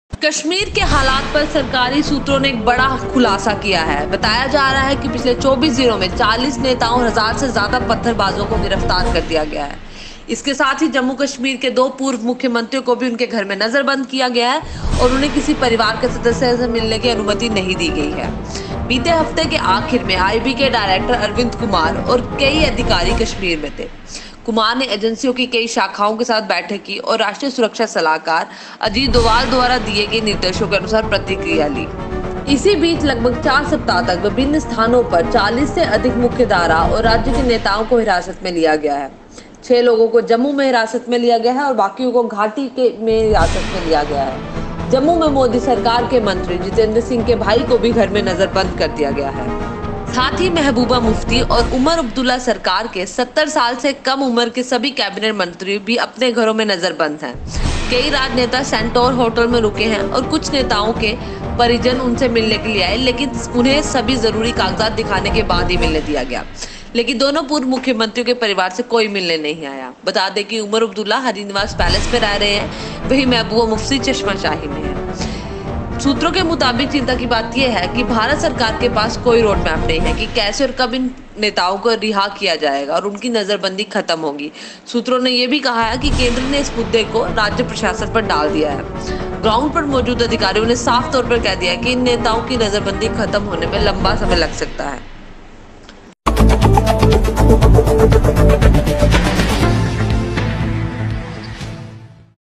Major Revelation on Kashmir Issue
Major Revelation on Kashmir Issue, Over 1000 stone-pelters arrested in last 24 days.